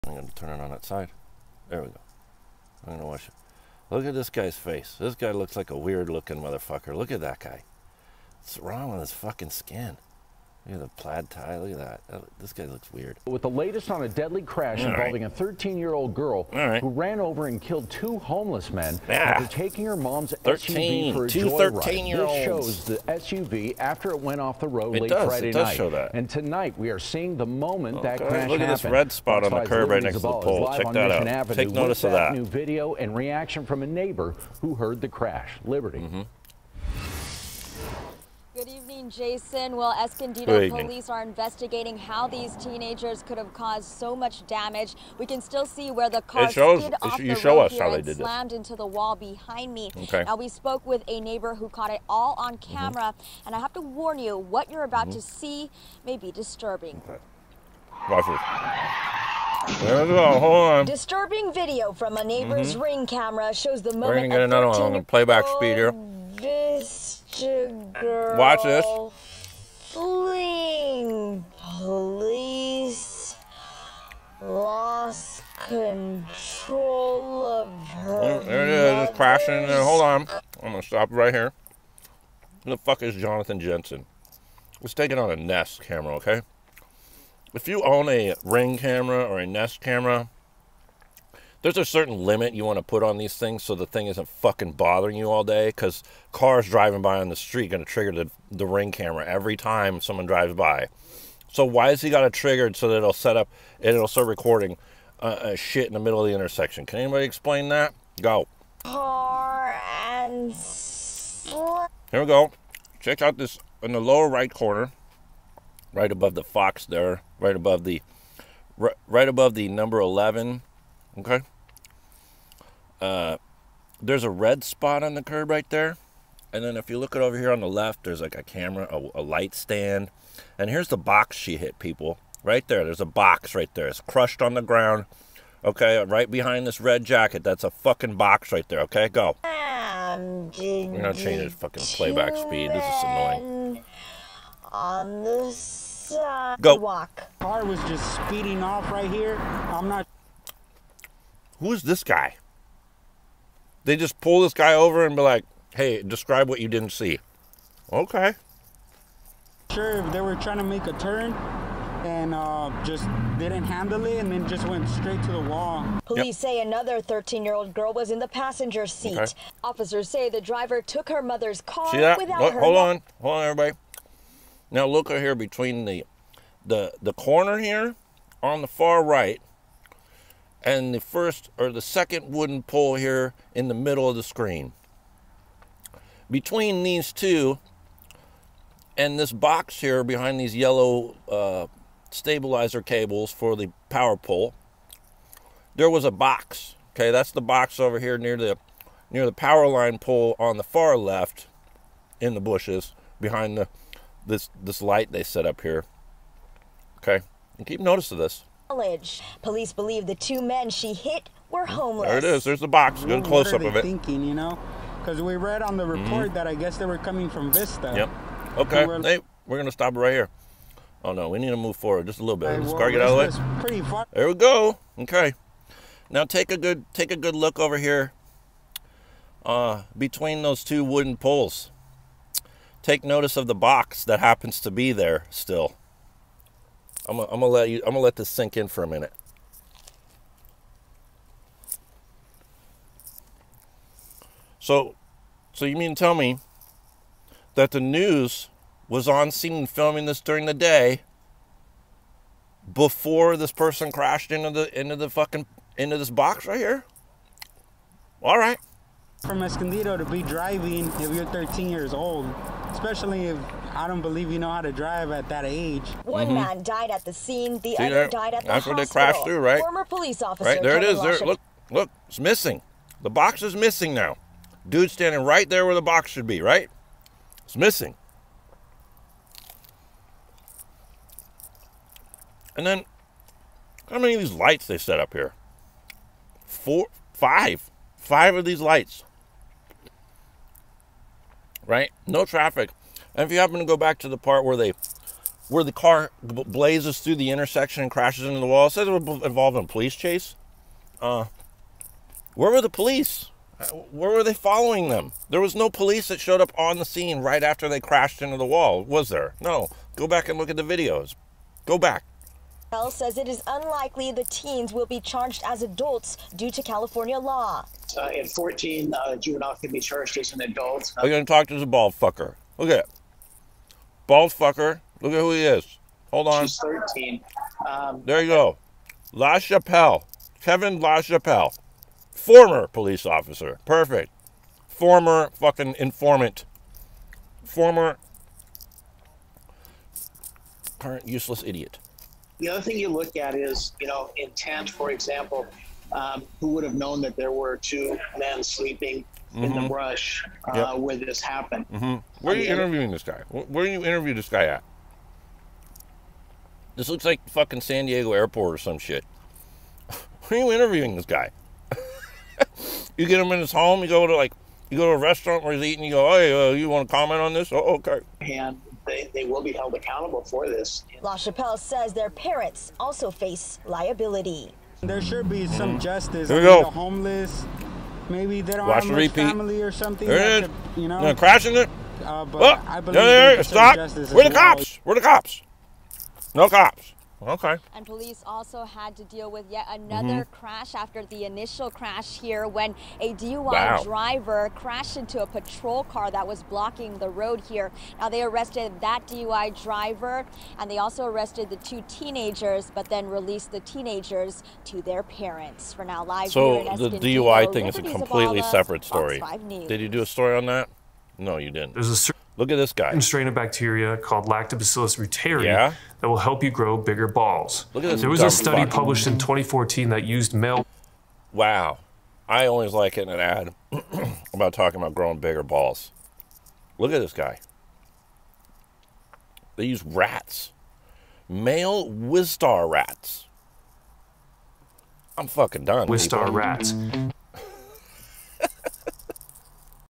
They might be psychic
The ever informing news, films a crash location before the crash. What are the odds?